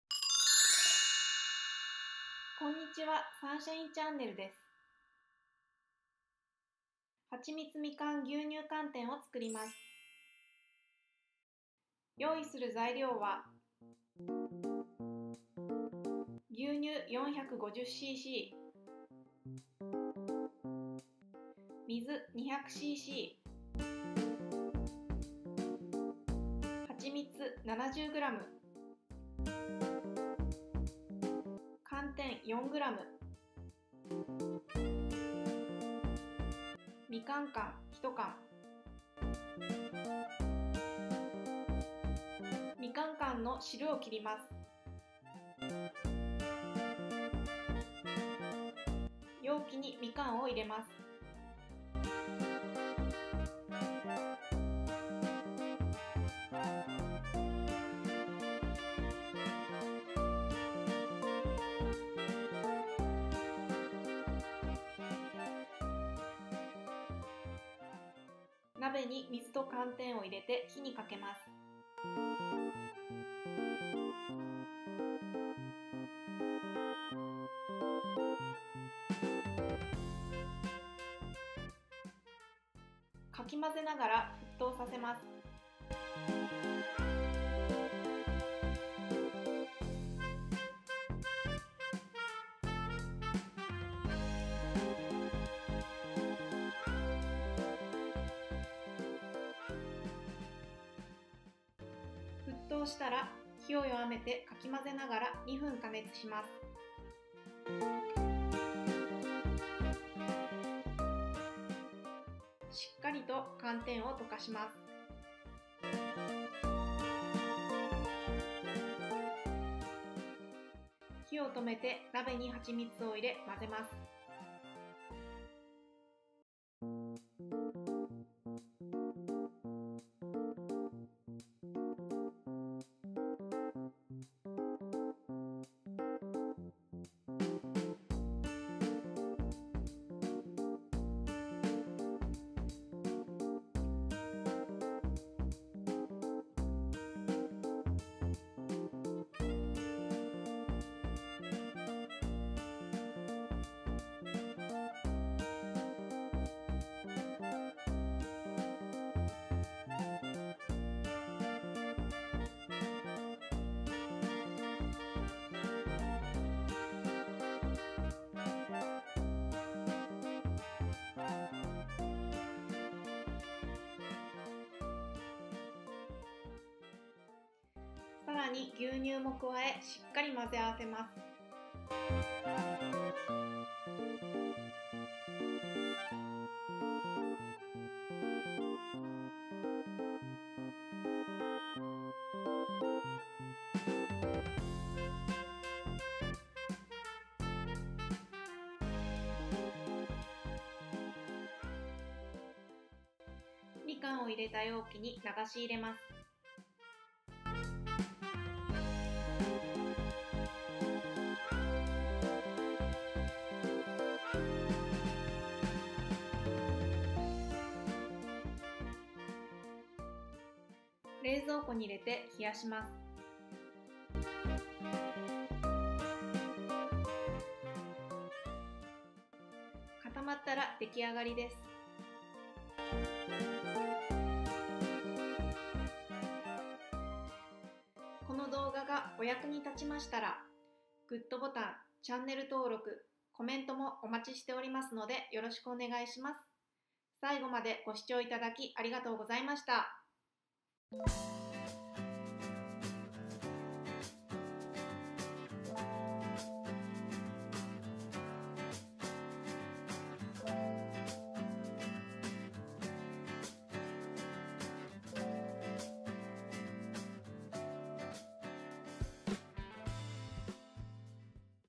【簡単デザート】はちみつみかん牛乳寒天の作り方
材料
牛乳　450cc
水　200cc
みかん缶　１缶
はちみつ　70g
寒天　4g

作り方
みかん缶の汁を切ります
容器にみかんを入れます
鍋に水と寒天を入れて火にかけます
かき混ぜながら沸騰させます
沸騰をしたら、時々かき混ぜながら2分加熱します
しっかりと寒天を溶かします
鍋にはちみつを入れ混ぜます
さらに牛乳も加えしっかり混ぜ合わせます
味見をして甘さを調整します
みかんを入れた容器に流し入れます
冷蔵庫に入れて冷やします
固まったら出来上がりです